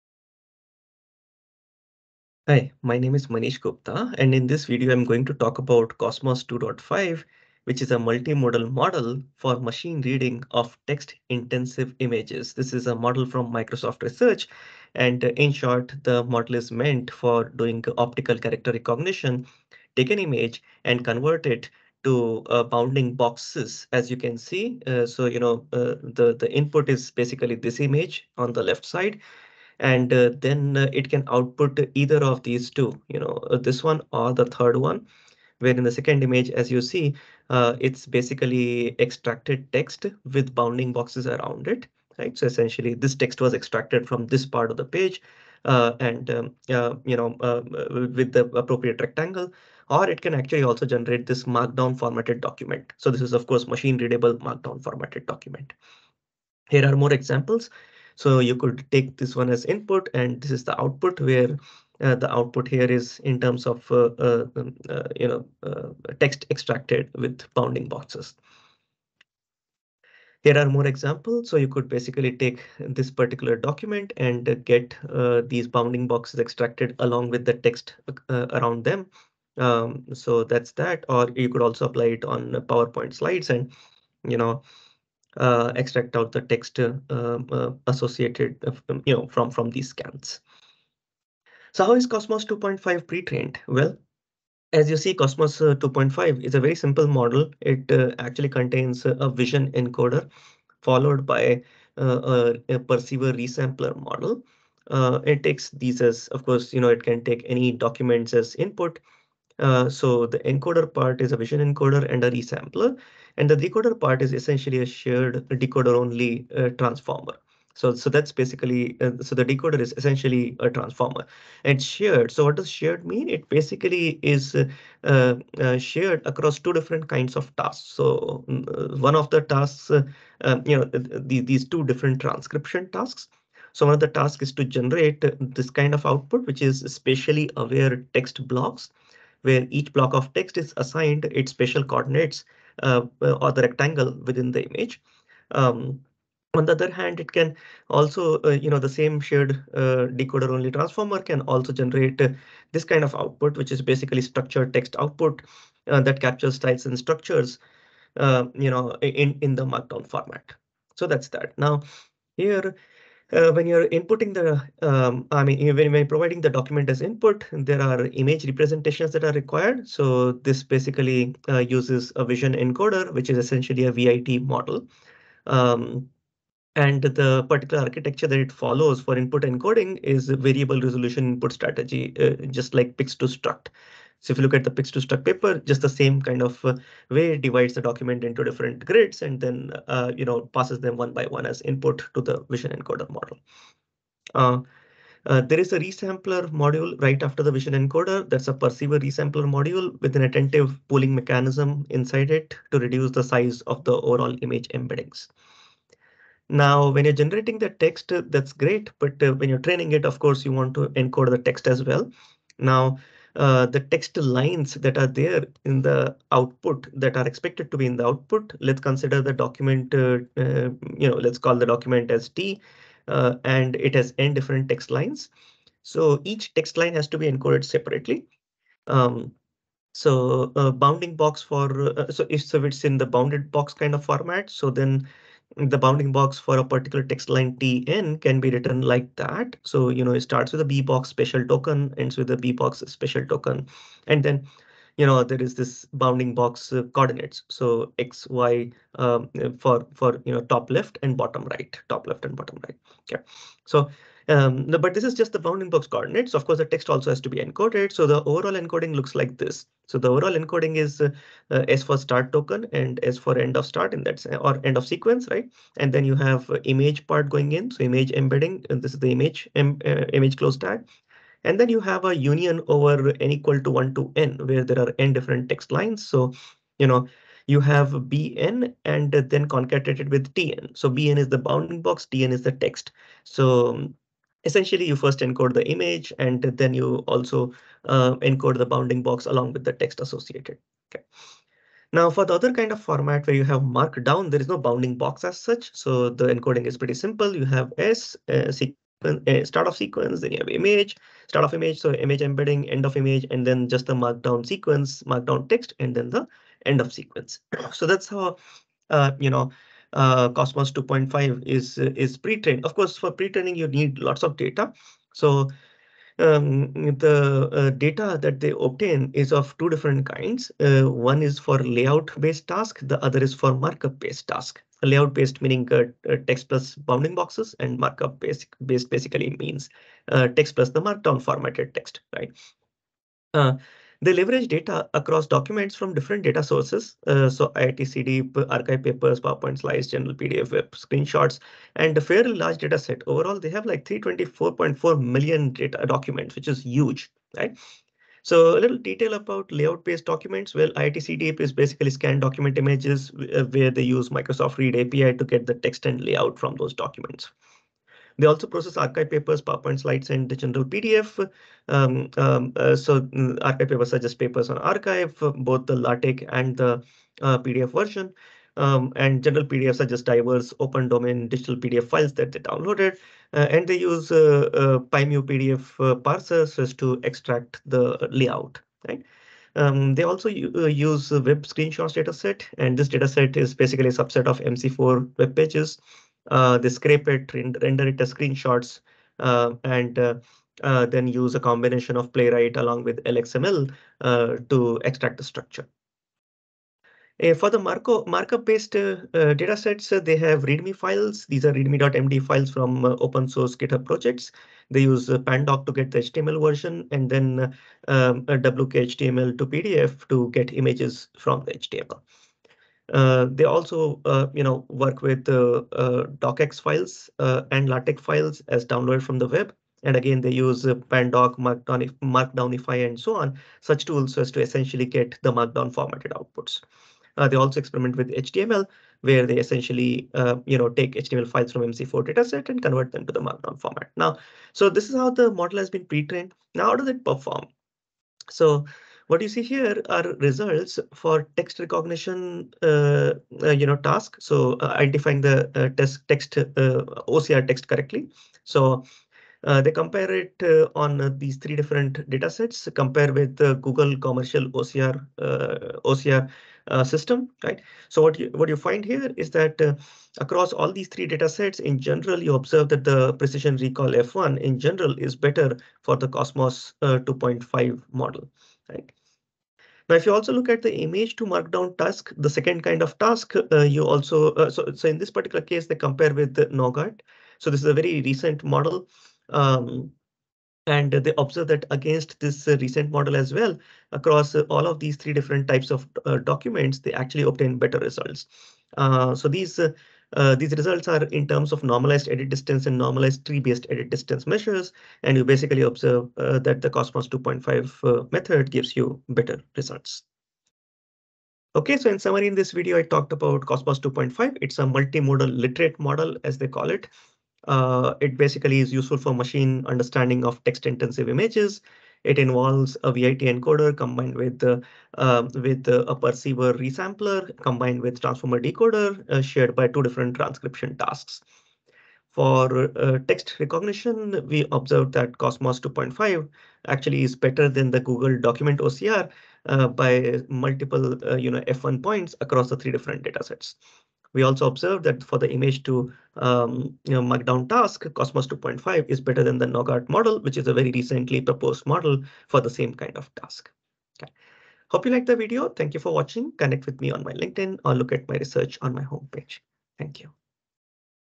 Microsoft's KOSMOS-2.5: A Multimodal Model for image to markdown
KOSMOS-2.5 is a multimodal literate model from Microsoft Research for machine understanding of text-intensive images. It consists of a ViT encoder combined with Perceiver Resampler and a Transformer decoder shared by 2 transcription tasks.
For text recognition, KOSMOS-2.5 outperforms Google Document OCR by 0.33%, 2.45%, and 1.35% F1.
For image-to-markdown task, KOSMOS-2.5 outperforms Nougat.

In this video, I will talk about the following: What is Machine Reading of Text Intensive Images? How is Kosmos-2.5 pretrained? How does Kosmos-2.5 perform?

KOSMOS-2.5: A Multimodal Literate Model. Tengchao Lv, Yupan Huang, Jingye Chen, Lei Cui, Shuming Ma, Yaoyao Chang, Shaohan Huang, Wenhui Wang, Li Dong, Weiyao Luo, Shaoxiang Wu, Guoxin Wang, Cha Zhang, Furu Wei. Microsoft. arxiv, Sep 2023.